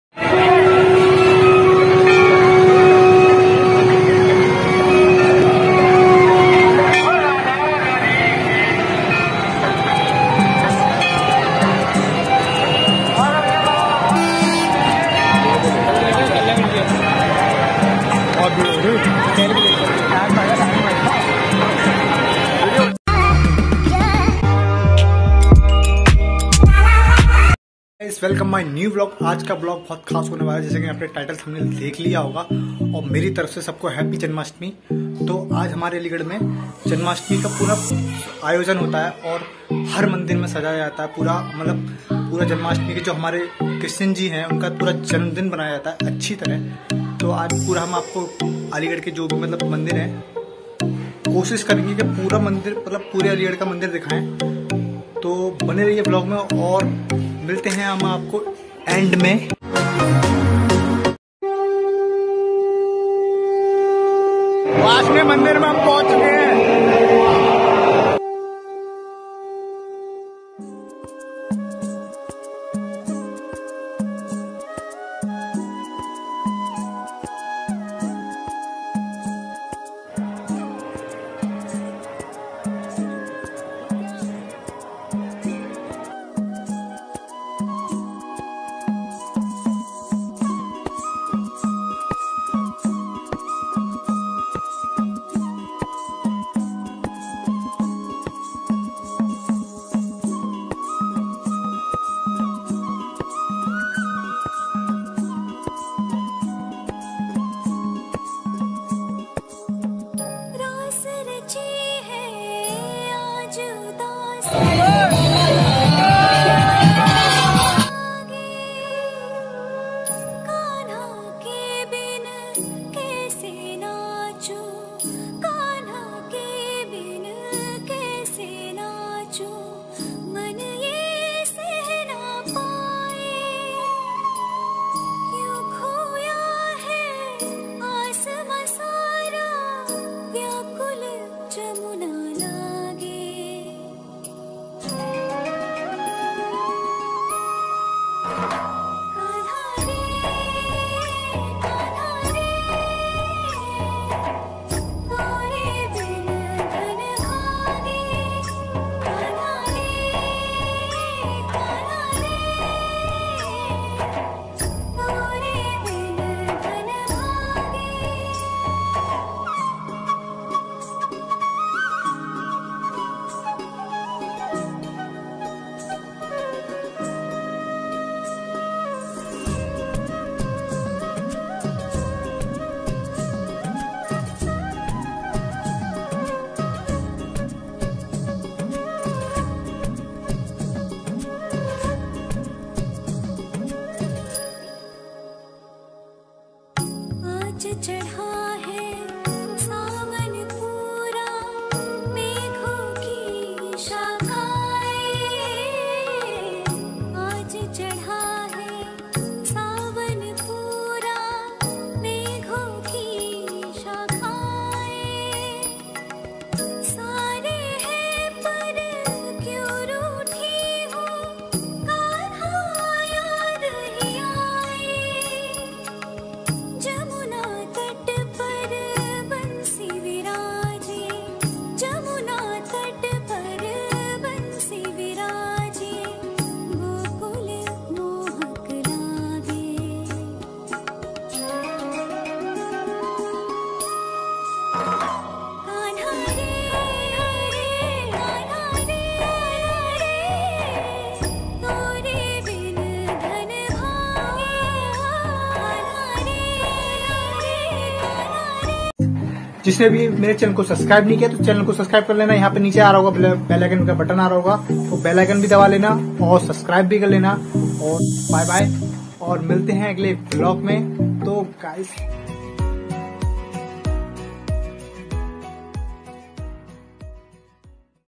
Aligarh me live janmastmi vlog🙏🙏🙏
Hy gyess walcome my new vlog aj h janmastmi mera taraf se happy janmastmi or aj apko Aligarh ke sab mandir ke drasan kraye ge bne rahiye vlog me 🙏🙏🙏

janmastmi vlog
sree radhey krishna
aligarh vlog
vlog

Byy gyess milte h agle vlog me 🙏🙏🙏🙏